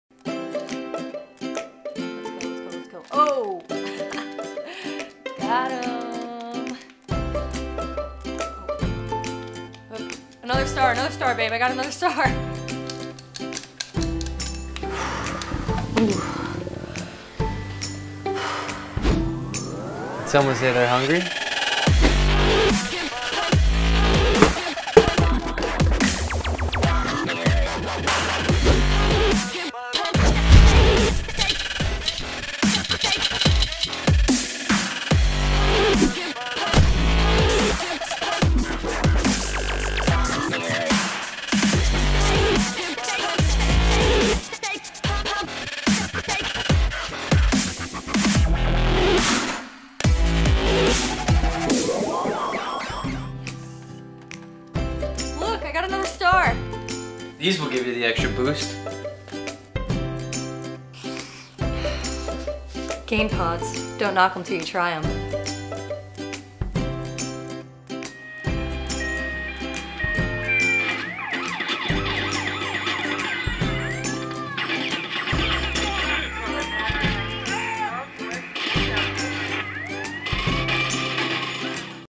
DID SHE REALLY JUST DO THAT? GAMER GIRL EATS GAIN PODS
Larks girlfriend gets pranked!  This couple prank goes terribly wrong because now she's addicted to eating Gain Pods... oh my god!! Prank gone wrong!  How will Lark ever recover from this mistake?